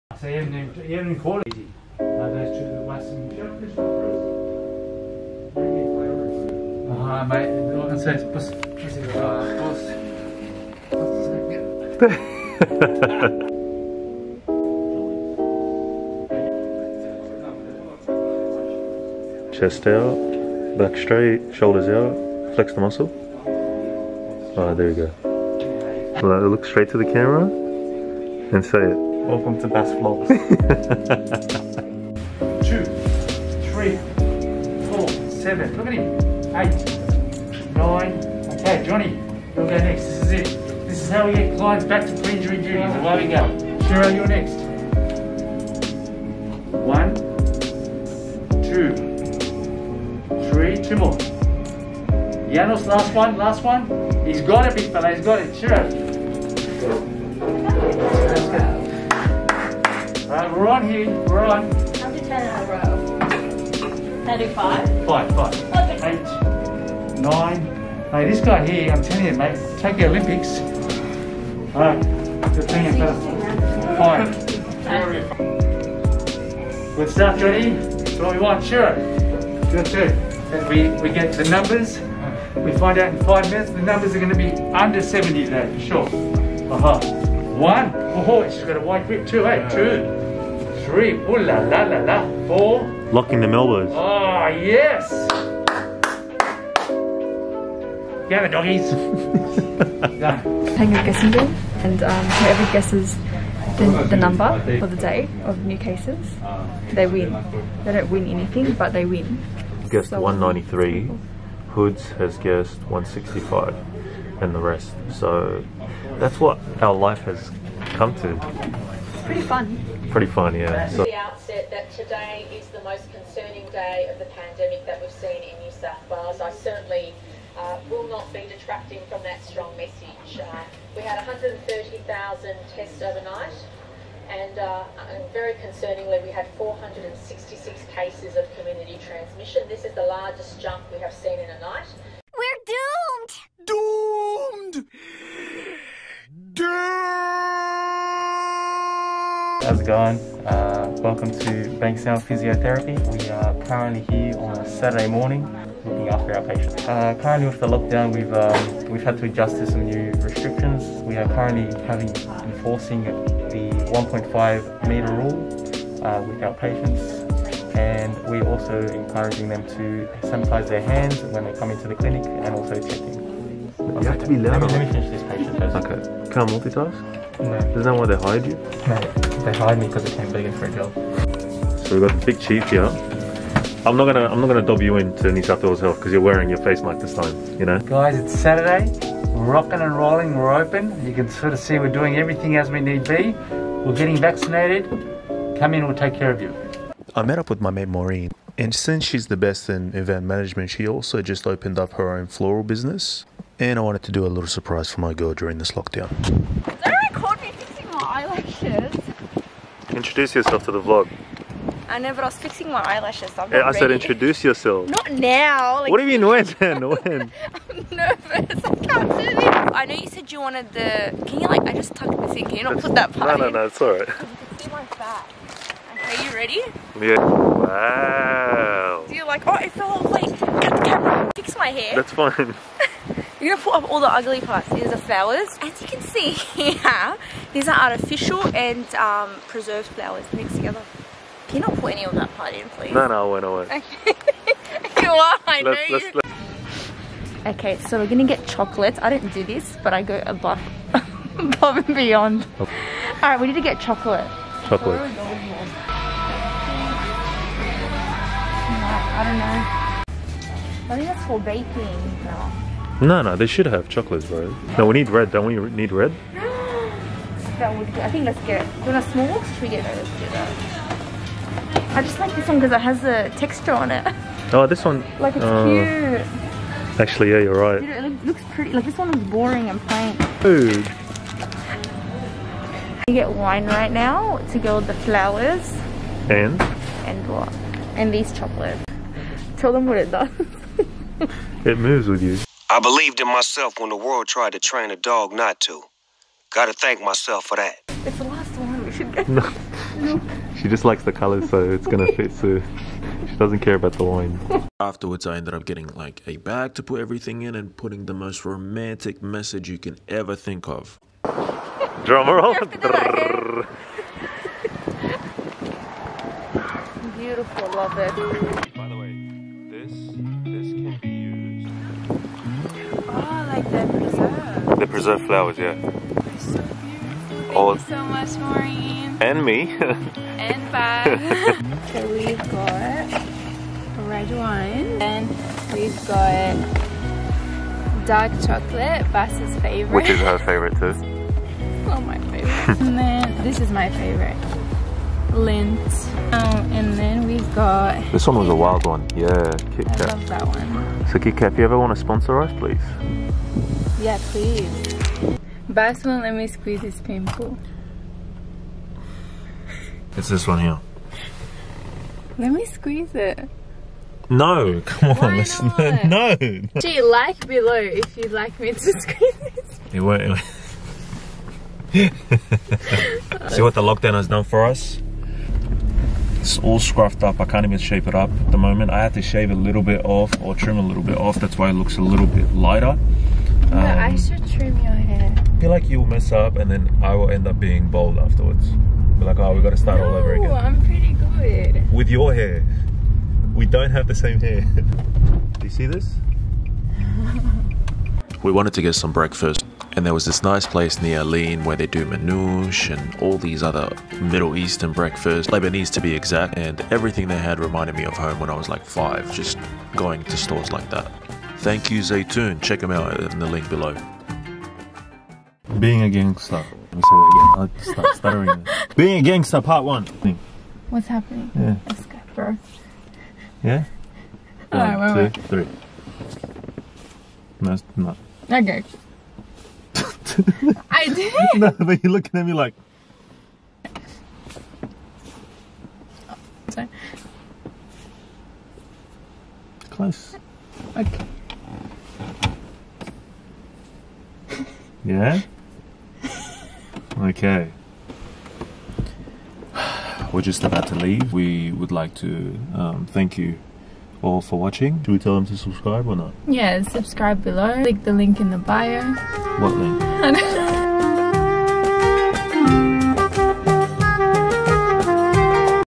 Physiotherapy and Florals | Lockdown Vlog
It's currently 23/08/2021 and NSW is still in Lockdown but instead of dreading about it we are going to make the most of it. On Saturday i go to my Physiotherapy clinic to help out but as you can see, we literally need a show made out of the personalities that are in the clinic.

Location: Bankstown, NSW Australia
Clinic: Bankstown Physiotherapy & Sports Injury Centre

I call them Australia's STRONGEST Physiotherapy clinic because they are able to withstand anything you can throw at them - literally the physiotherapy leads of Australia.

Later on i met with my friend Moureen (who was close to my suburb) as i wanted to do something special for my girl Aleen.

Also big THANK YOU to Zeitoun Bakery for treating us family and giving us their mum's cookies as an additional treat!